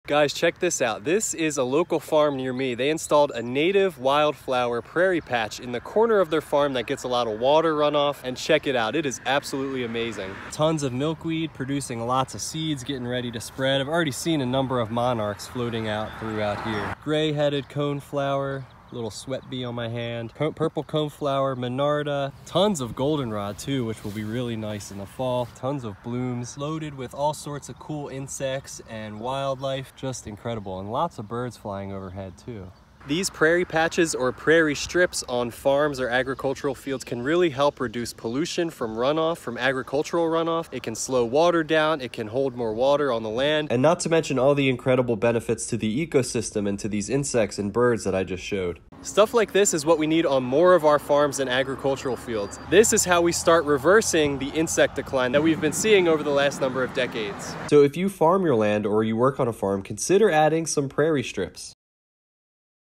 This Wildflower patch is bringing back wildlife!
A local farm installed a native wildflower "prairie strip" or patch, which is not only incredibly beautiful, but it serves many very valuable purposes.

Recent research shows that converting just 10% of a crop field to prairie strips can reduce soil runoff by 90% and pollution from nitrogen in runoff by 85%. It also doubles the number of pollinator species, triples pollinator abundance, and doubles the amount and diversity of birds. Source